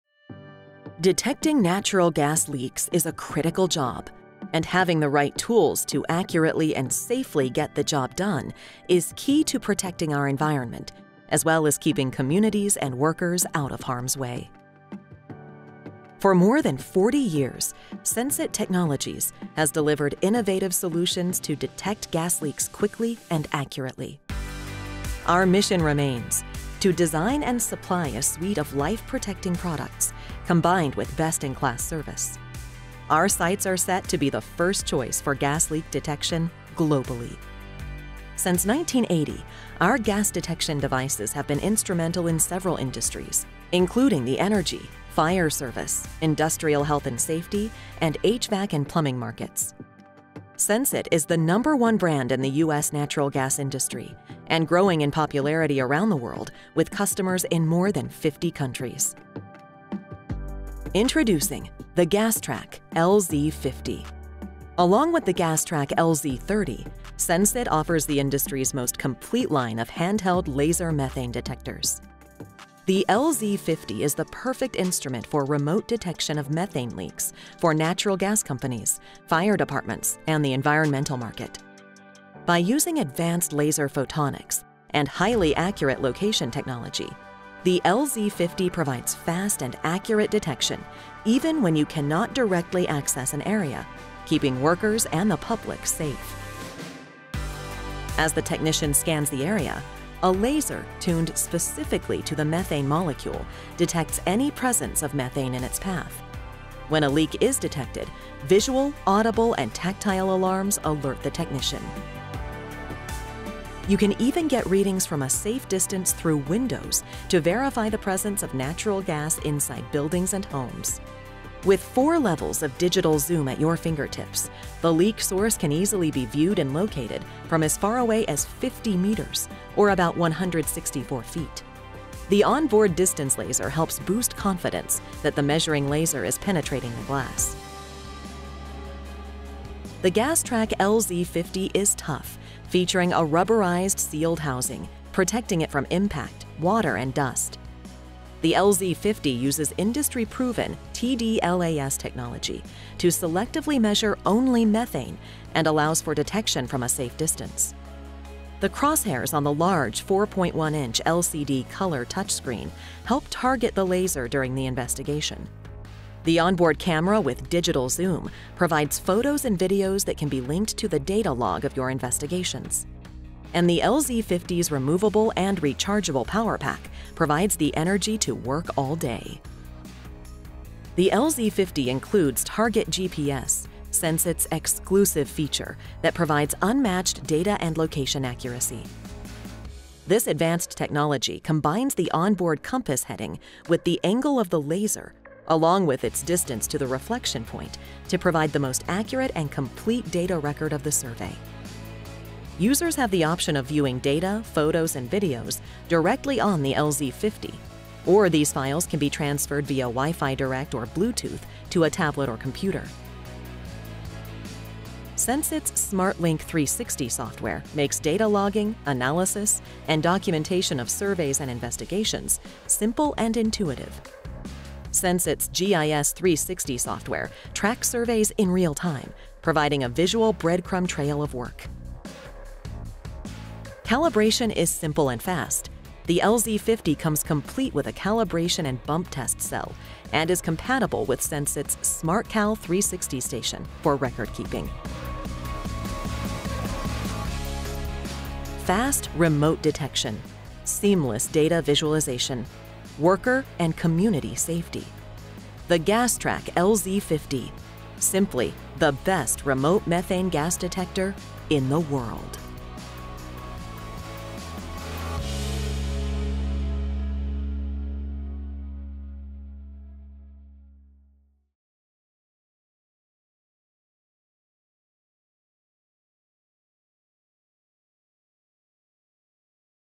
GAS TRAC® LZ-50 Introduction
The GAS•TRAC® LZ-50 from SENSIT Technologies is a compact methane-specific leak detector that provides fast and accurate readings at distances up to 164 feet.

The LZ-50 utilizes TDLAS (tunable diode laser absorption spectroscopy) technology to provide instantaneous and accurate responses to the presence of methane gas.

Designed to be competitively priced, the LZ-50 is perfect for hard to reach areas, eliminating the need to directly access the gas plume, climb to elevated areas, or access fenced-in property, or other potentially hazardous situations.

Quickly and safely detect methane gas leaks in difficult to reach areas with the GAS-TRAC® LZ-50.